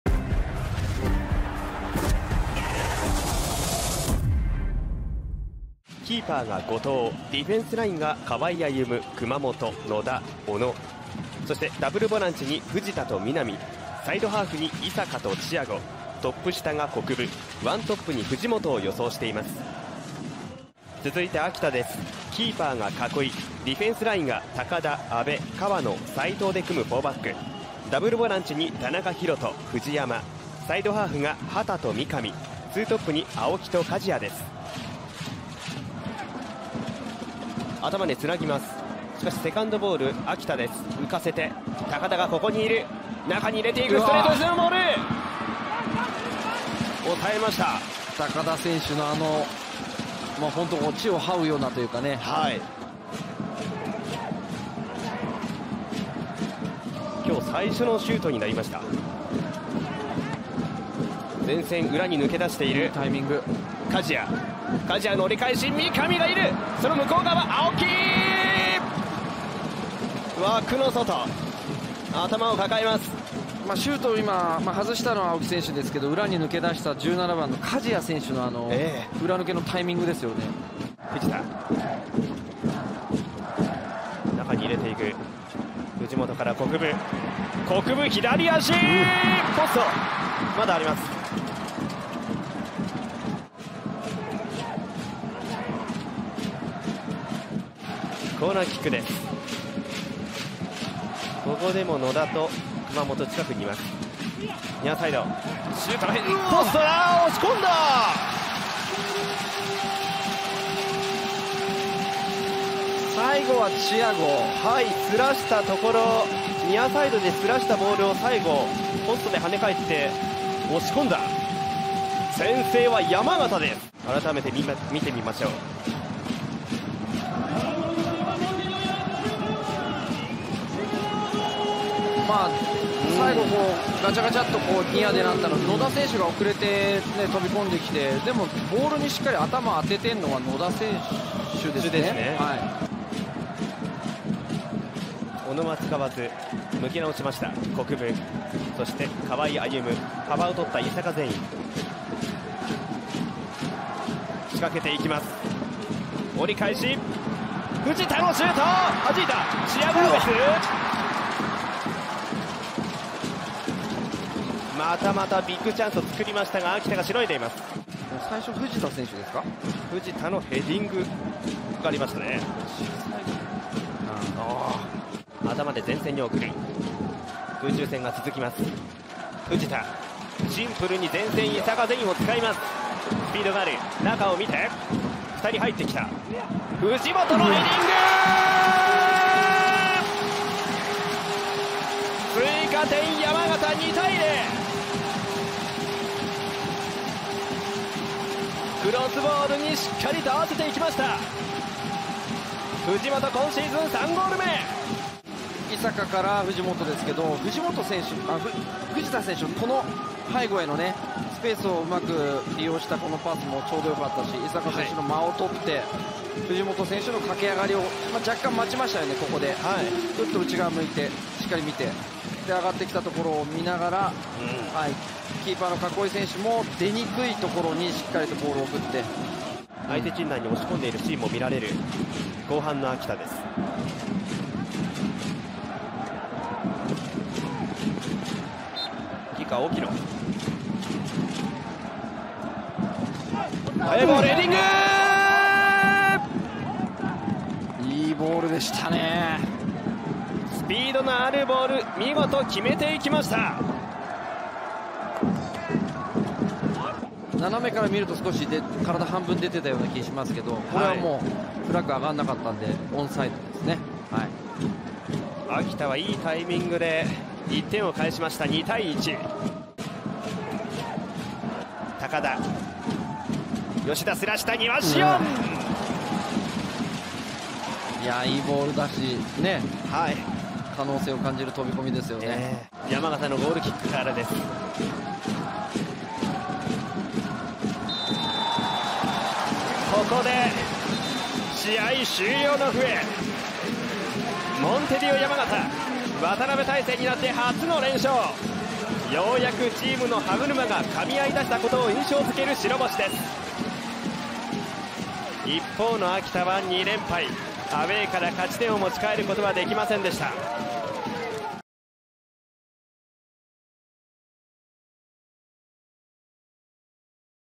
【公式ハイライト】モンテディオ山形 vs ブラウブリッツ秋田（2023明治安田生命J2リーグ 第17節）
2023明治安田生命J2リーグ 第17節
モンテディオ山形 2-1 ブラウブリッツ秋田
2023年5月21日14時キックオフ／ NDソフトスタジアム山形
[得点者：54' 畑潤基]

▼試合結果・写真・監督コメントはこちら！

▼Jリーグ全試合配信！DAZNへのご加入はこちら！